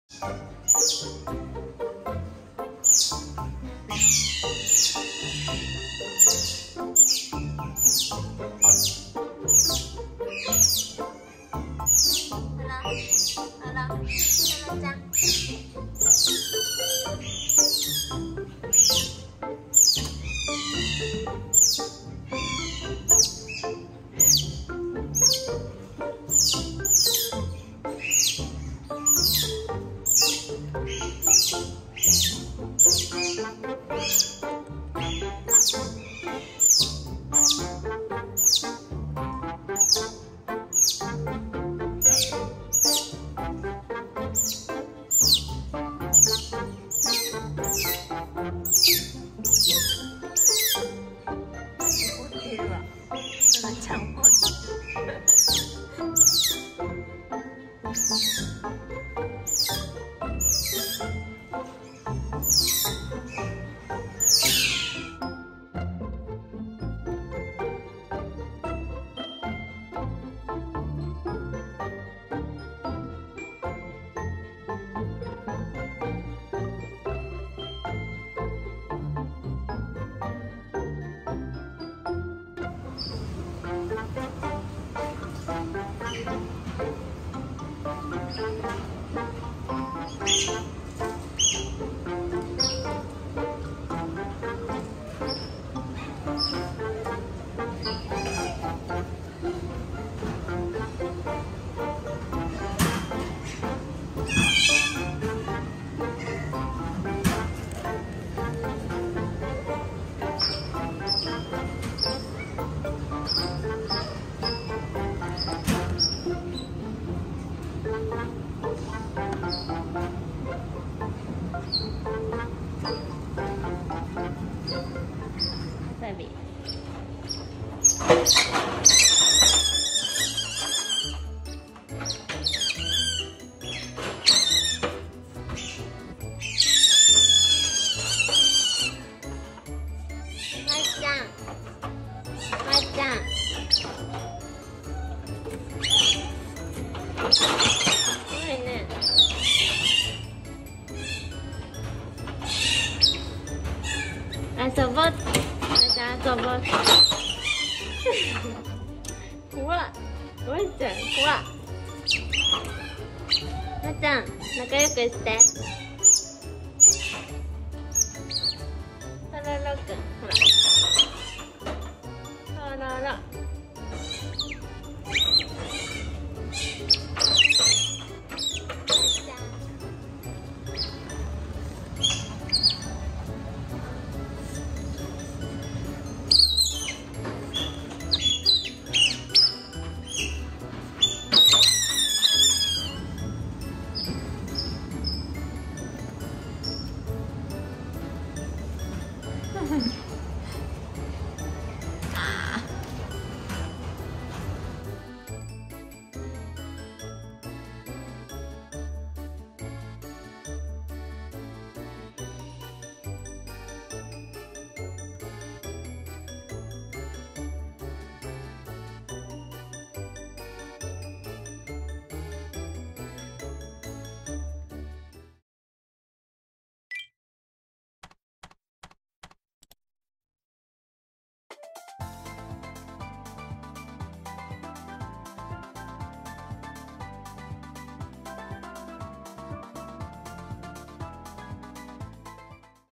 Baby Otter Met His Sisters
Otter Introduction
Tororo : Baby Otter, 3 months old
Wasabi & Namida : Tororo's old sister, 1.5 years old